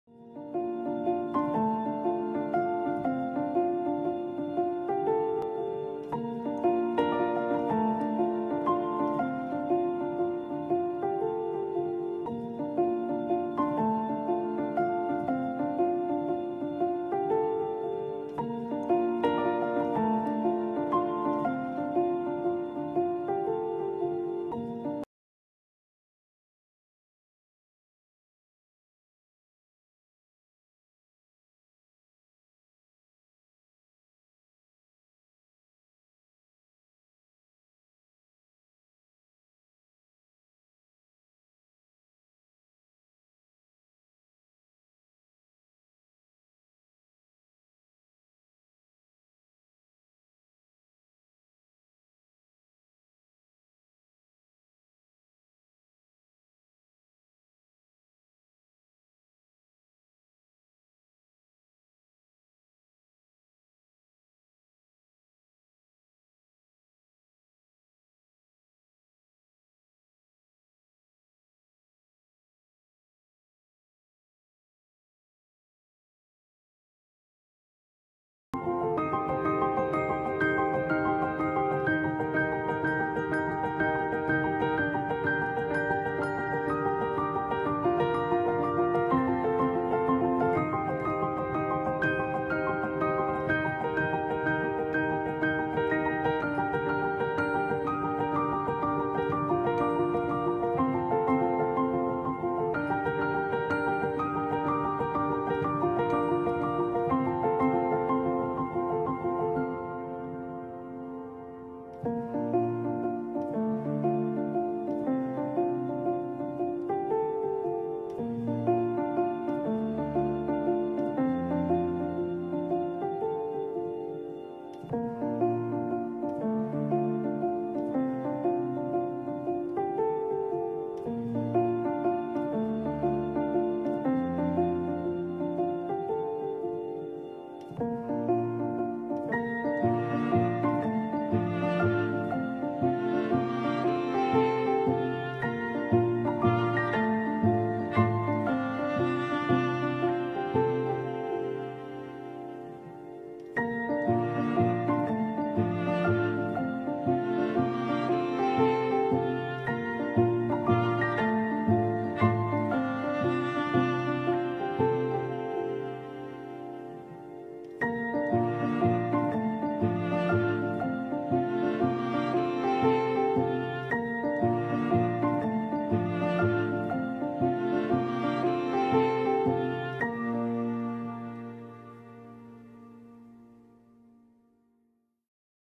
[ Sarcocaulon Multifidum ] 植物界的網美 - 月界 (黑皮月界) | 多肉植物 塊根植物 Succulent Caudex
Youtube較少黑皮月界的影片, 所以拍了這個, 順便作紀錄❤️